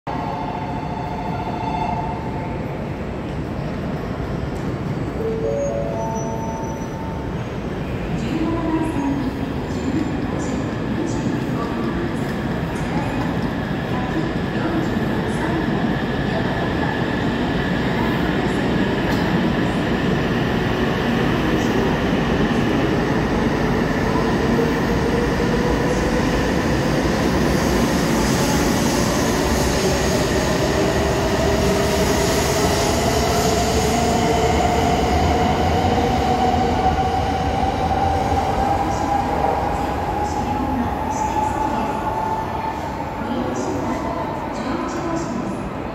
JR東日本新幹線E2系1000番台J66編成『国鉄200系復刻塗装』が回送列車として大宮駅18番線を発車するシーン 2023.2.12
【まさかの国鉄200系カラーが来た】JR東日本新幹線E2系1000番台J66編成『国鉄200系復刻塗装』が回送列車として大宮駅18番線を発車するシーン(列番不明) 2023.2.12

↑正式なタイトル

銀色つばさ復刻塗装の練習電として偶然来ました
初めて新幹線撮ったのがこれだったのでいい思い出になりました。

追記
今日から試しに正式なタイトルを概要欄に書くことにして見ました。これでどうかな🤔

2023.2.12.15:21